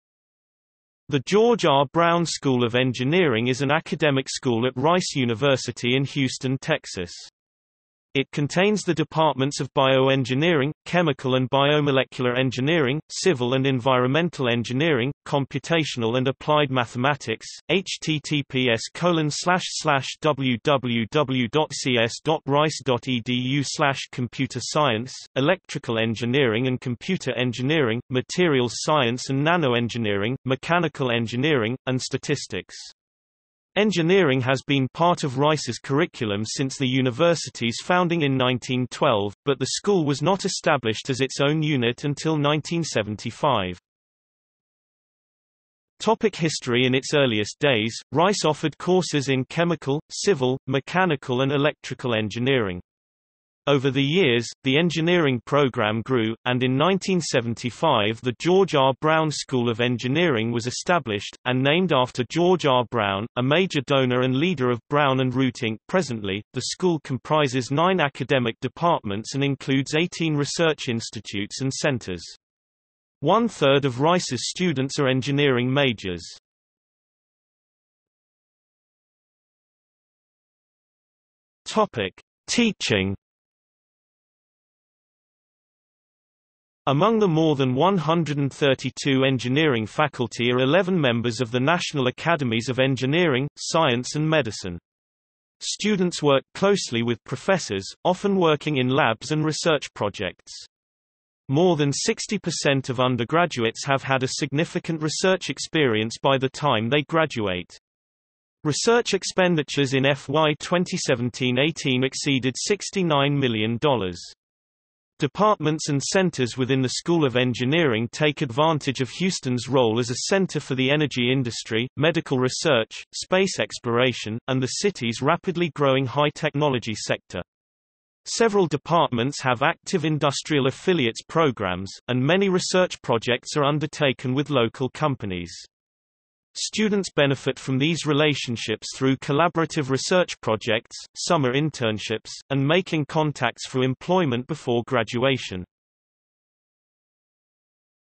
George R. Brown School of Engineering | Wikipedia audio article
00:00:45 1 History
00:01:18 2 Teaching

- Socrates

SUMMARY
The George R. Brown School of Engineering is an academic school at Rice University in Houston, Texas. It contains the departments of Bioengineering, Chemical and Biomolecular Engineering, Civil and Environmental Engineering, Computational and Applied Mathematics, Computer Science, Electrical Engineering and Computer Engineering, Materials Science and Nanoengineering, Mechanical Engineering, and Statistics. Engineering has been part of Rice's curriculum since the university's founding in 1912, but the school was not established as its own unit until 1975.